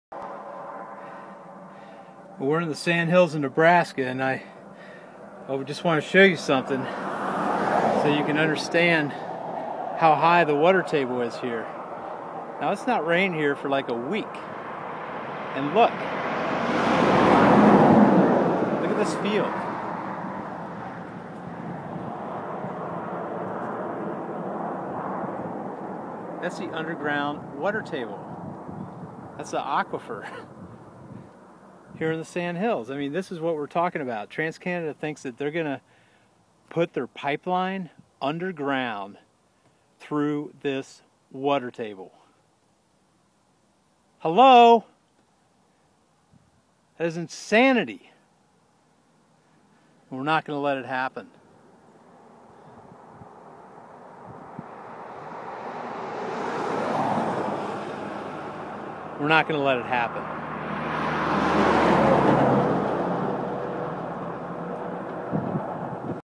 Ogallala Aquifer Exposed on Surface in Nebraska's Sandhills
Renewable Rider Tom Weis shows the Ogallala Aquifer exposed on the surface in the Sandhills region of Nebraska. He talks about how insane it would be to put a toxic tar sands pipeline underground in such a fragile area of the country.